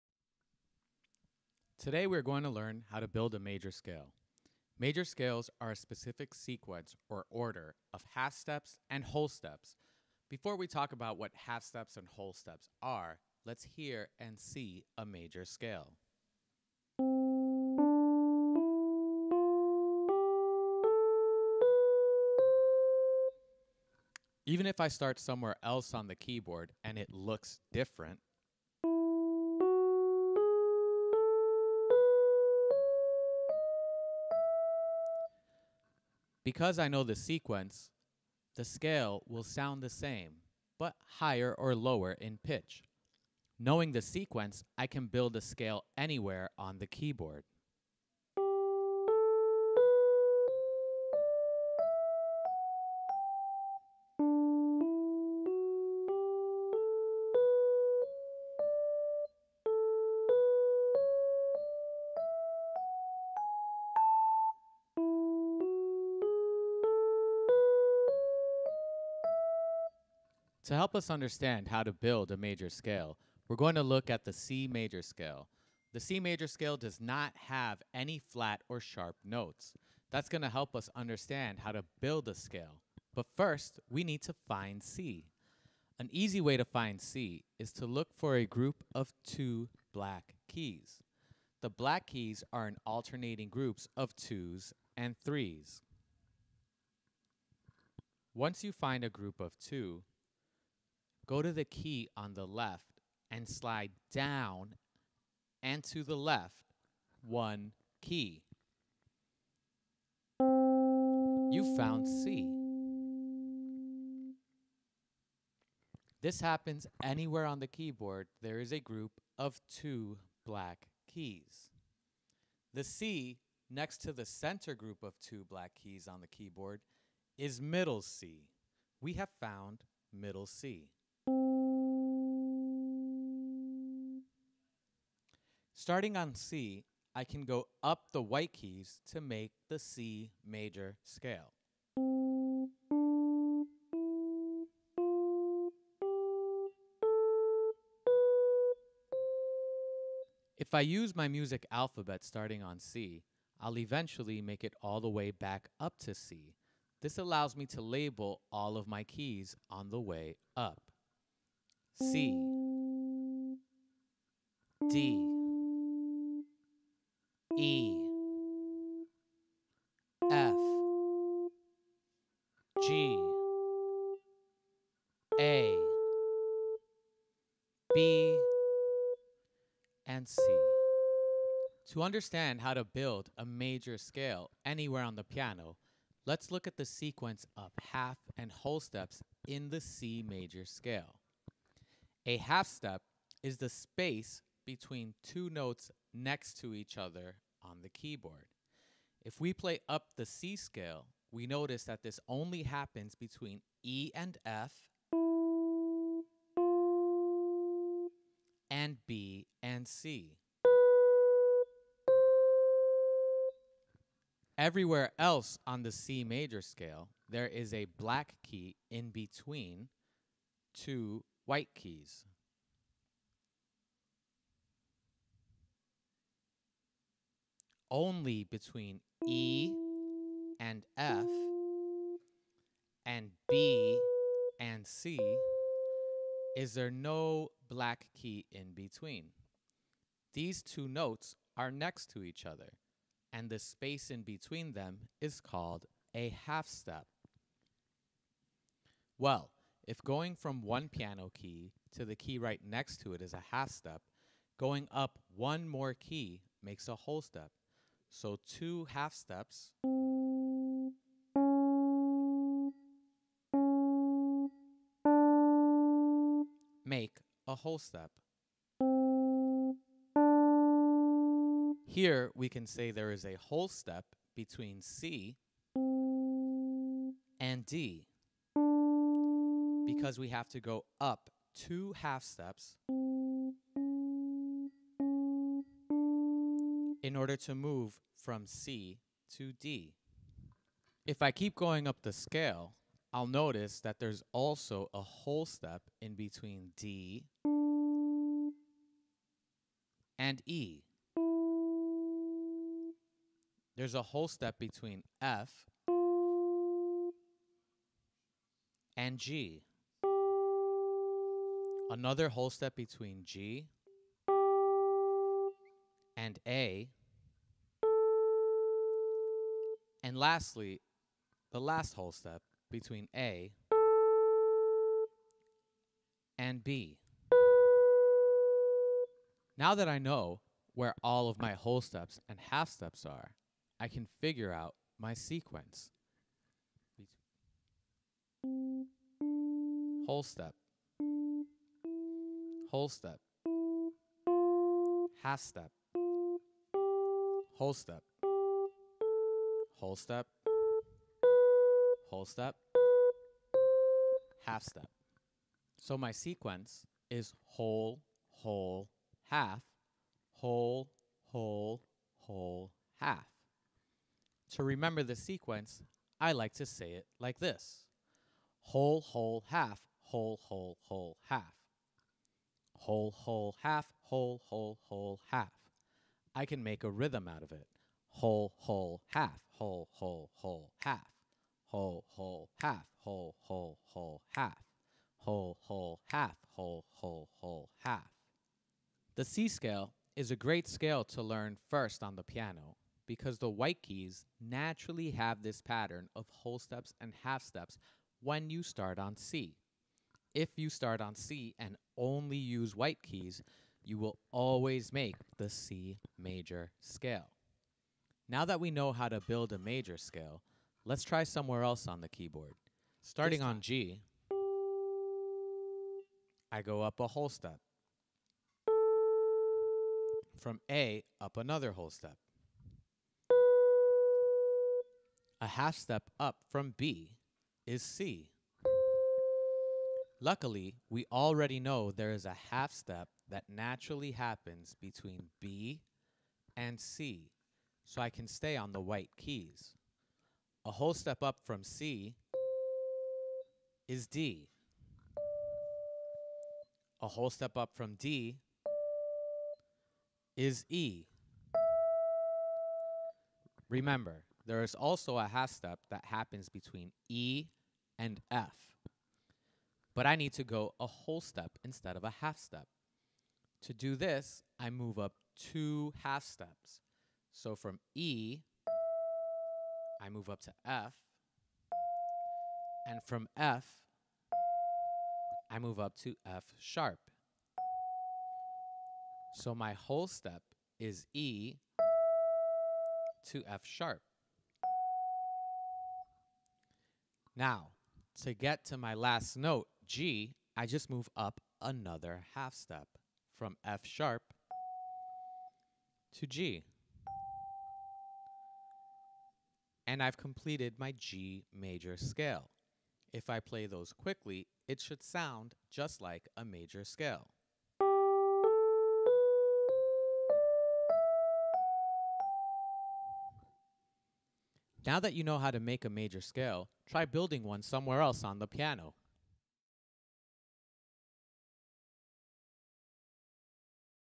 Music. How to Build a Major Scale. 3-8. Extra Enrichment.
Looking at the Keyboard, we will learn how to find middle C, learn the notes in the C Major Scale, find where the half steps and whole steps are located in a Major Scale, use the sequence WWHWWWH to make a G Major Scale.